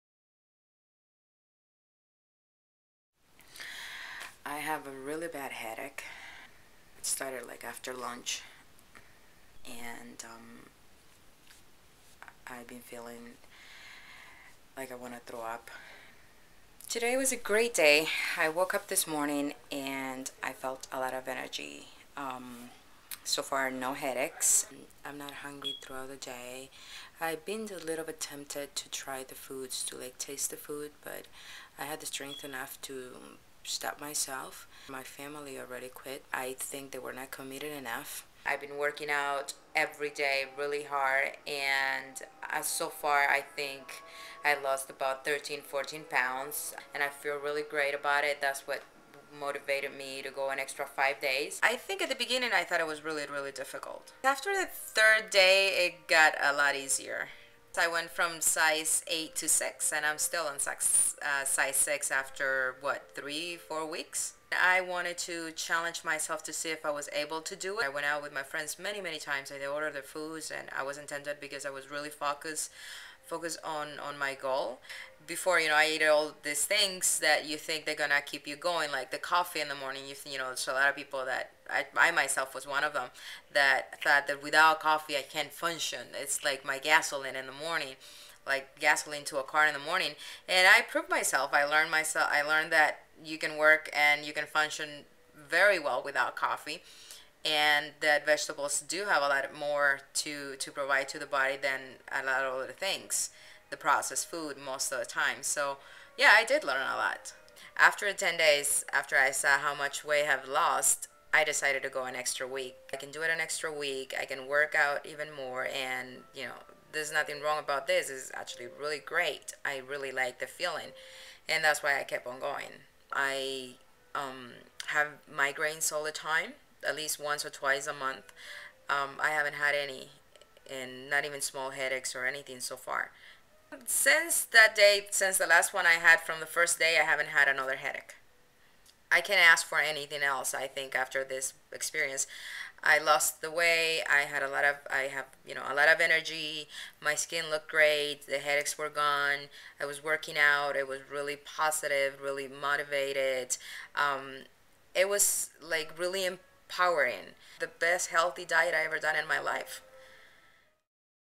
Catalina's Reboot Success Story
Catalina set out to try a 10 day Reboot; she felt so great and was having such positive results that she decided to extend it to 15 days.

Watch Catalina transform before your eyes in this Reboot success story video!